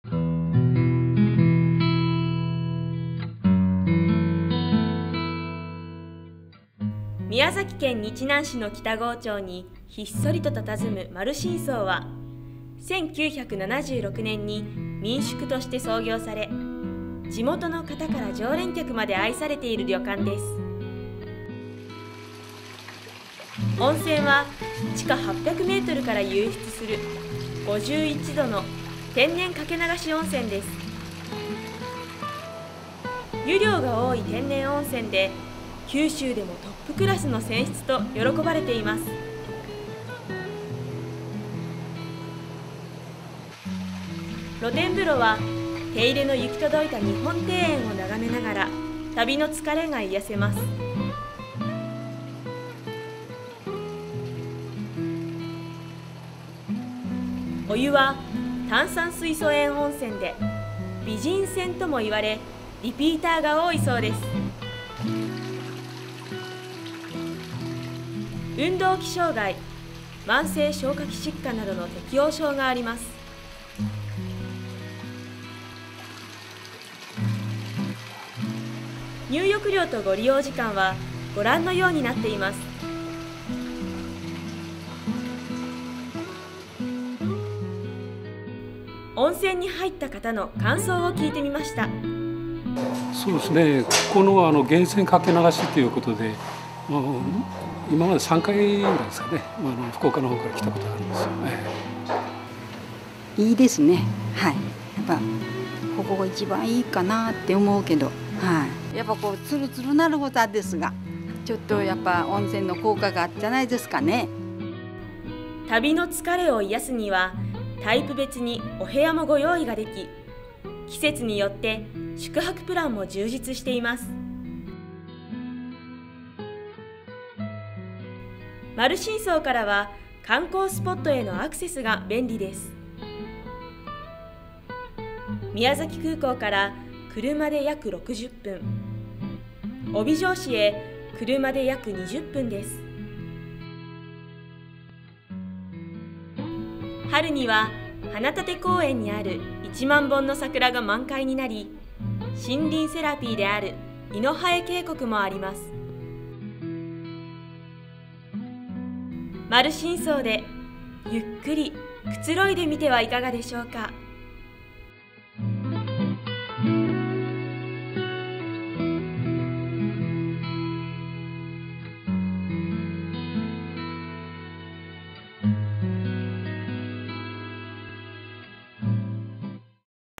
【日帰り温泉】すべて源泉掛け流しの丸新荘（宮崎県日南市）
宮崎県日南市の北郷町にひっそりと佇む「丸新荘」は、１９７６（昭和５１）年に民宿として創業され、地元の方から常連客まで愛されています。　温泉は地下８００ｍから湧出する５１℃の天然掛け流し温泉です。湯量が多い天然温泉で、九州でもトップクラスの泉質と喜ばれています。
　露天風呂（女湯）は、手入れの行き届いた日本庭園を眺めながら、旅の疲れが癒せます。　旅のタイプ別にお部屋もご用意ができ、季節によって宿泊プランも充実しています（お1人様1泊2食 平日7,500円〜）。
　春には花立公園にある１万本の桜が満開になり、森林セラピーである猪八重渓谷もあります。
丸新荘でゆっくり くつろいでみてはいかがでしょうか？